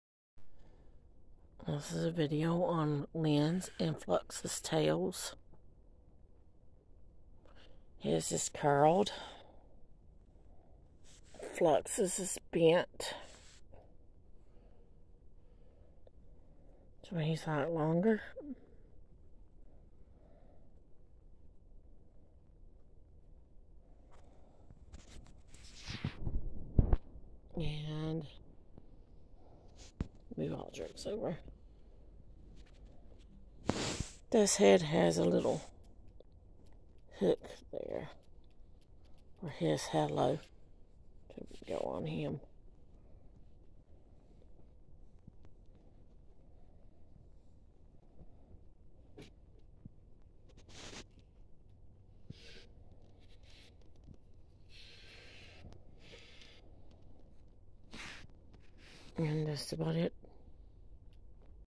LENS, Flux Tails and Death's hook
showing LENS and Flux's Tails and death's hook.
LENS and Flux are both Digidevils.
they all are my new custom plushies.